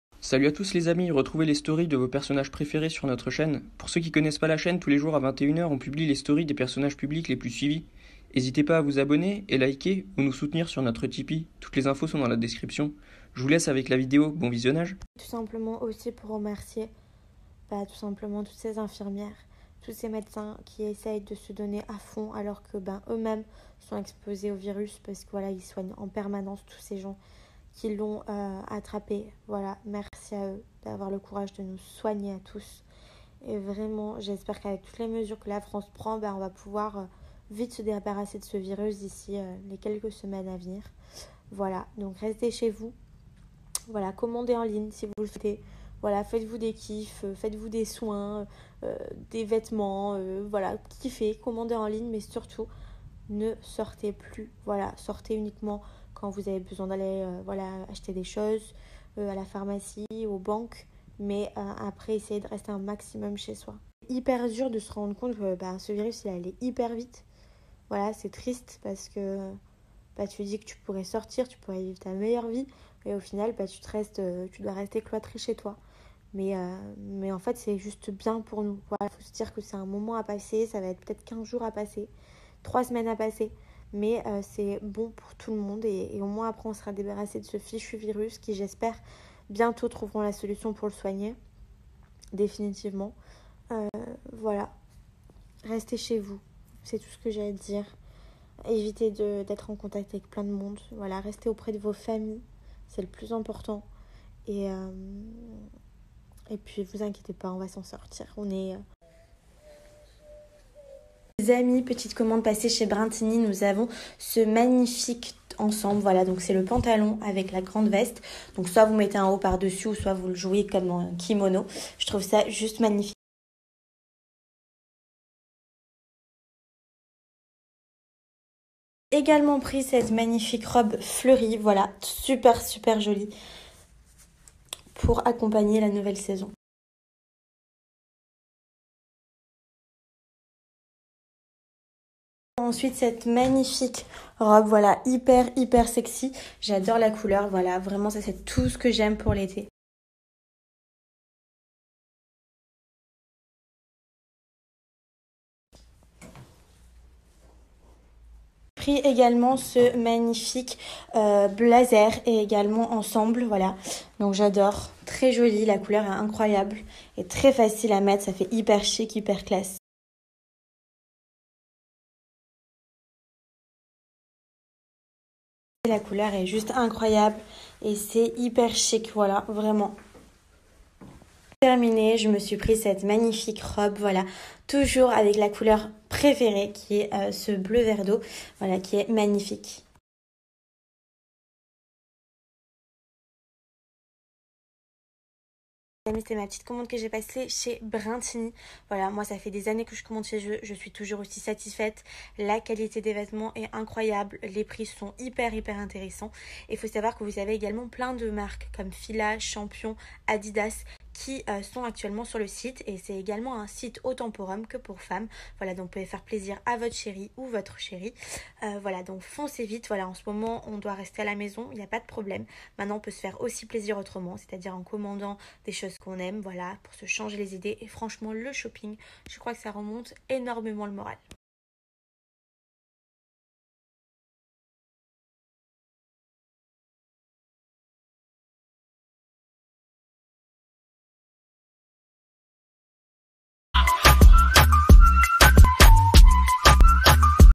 CARLA MOREAU 24H STORY INSTAGRAM - 15 Mars 2020
Revivez en intégralité les Story de Carla le 15 Mars 2020 - Pour plus de story consultez notre playlist Carla Moreau

Bon visionnage :)